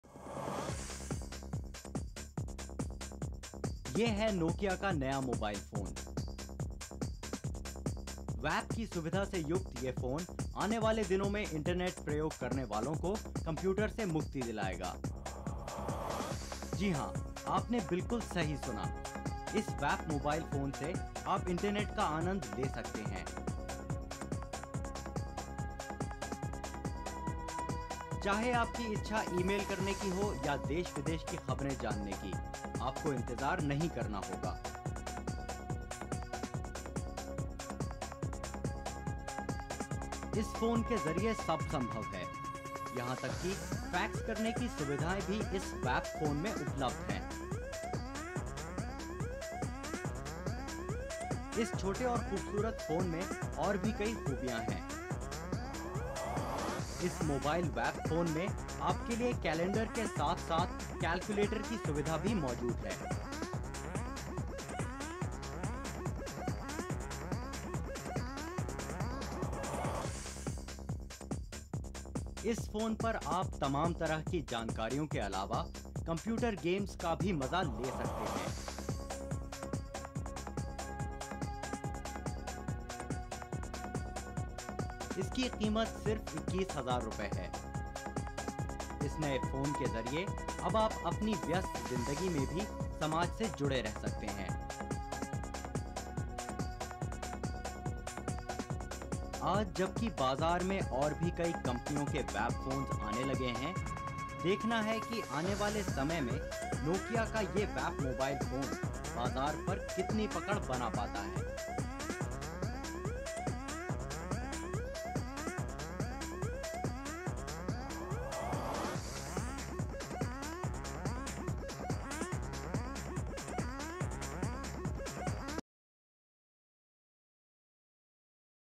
Nokia launches its first WAP enabled mobile phone in 1999
Nokia launches its first WAP or Wireless Application Protocol in India. This WAP feature gives you access to the internet through your mobile phone. Having internet service on your phone gives you accessibility to read news stories on your phone, emails, fax etc. This phone also has calculator and games feature.